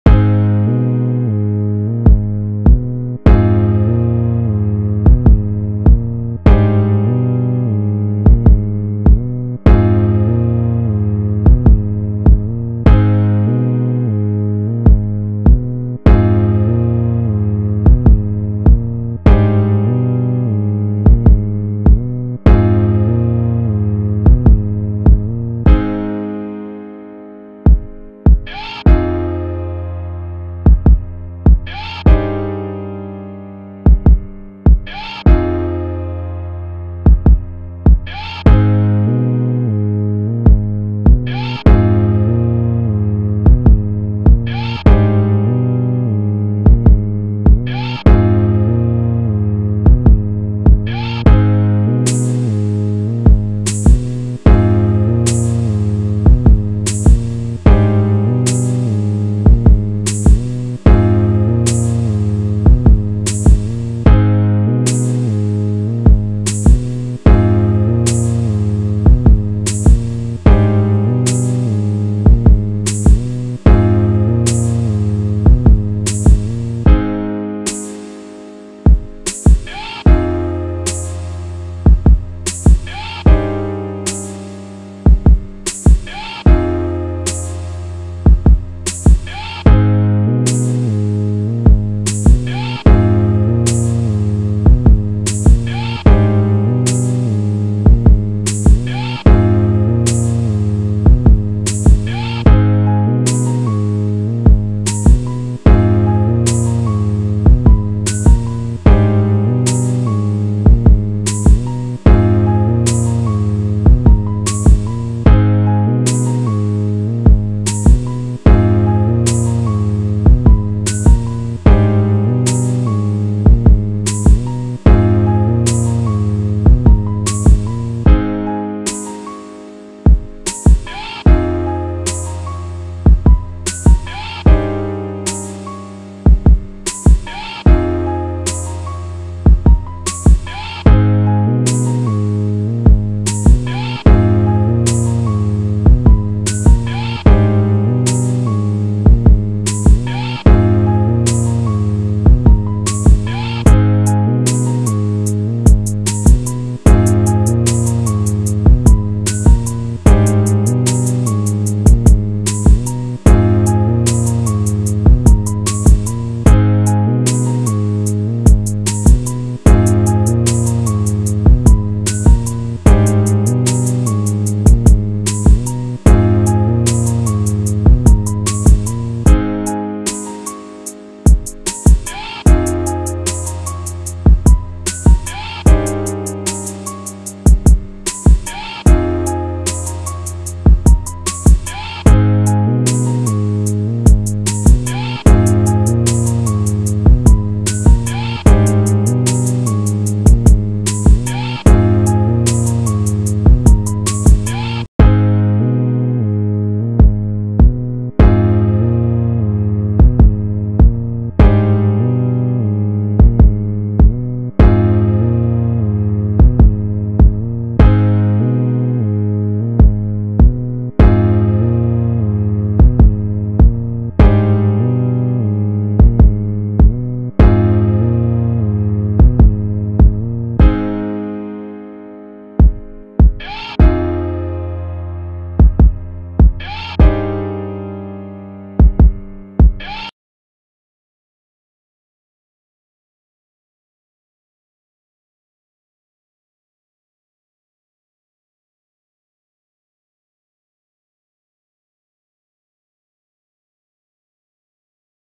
Realistic Nike Air Vapor Max speed art
In this video you are watching me create the Nike AIr Vapor Max from scratch. The realistic drawing of the Nike Air Vapor max speed art.